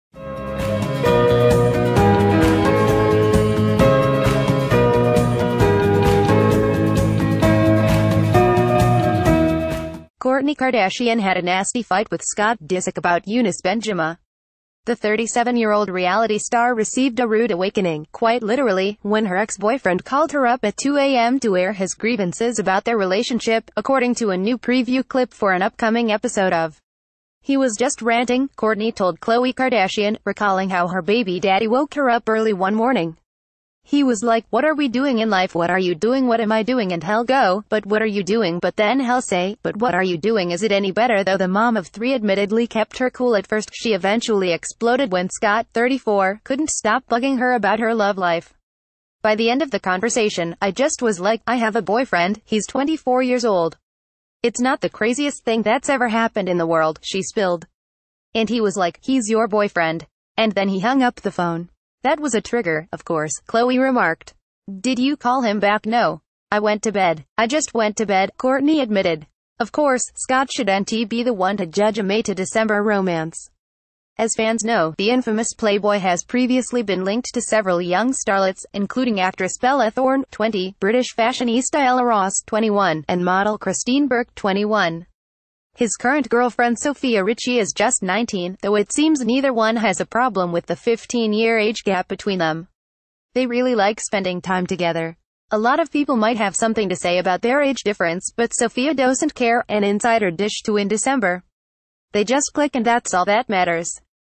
Kourtney Kardashian Scott Disick fight explosion leads to see stars of today
Kourtney Kardashian had a nasty fight with Scott Disick about Younes Bendjima. The 37-year-old reality star received a rude awakening — quite literally! — when her ex-boyfriend called her up at 2 a.m.